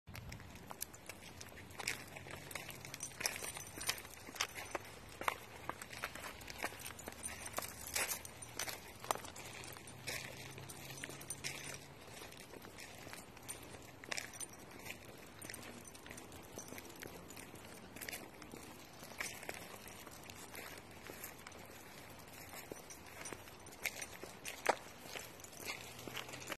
Video of adoptable pet named Smurfette
Smurfette is an adoptable pet with Underdog Rescue MN in St. Louis Park MN.